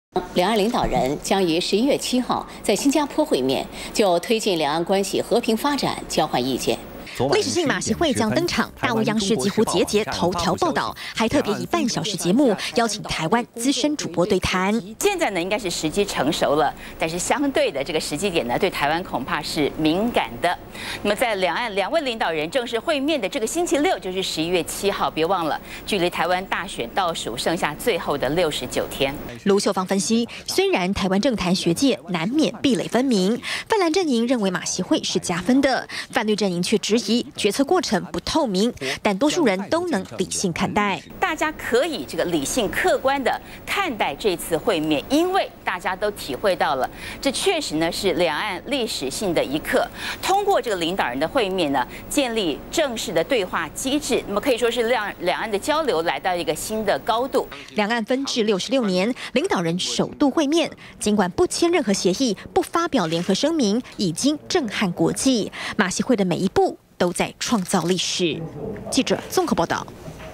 20151105中天新聞 央視頭條報馬習會 對談中天主播
馬習會本周六即將舉行，全球矚目，大陸央視重點報導，還以半小時節目，邀請中天資深主播盧秀芳對談，盧秀芳分析，大部分台灣民眾都能理性看待馬習會，通過領導人會面，能建立正式的對話機制，讓兩岸交流來到新高度。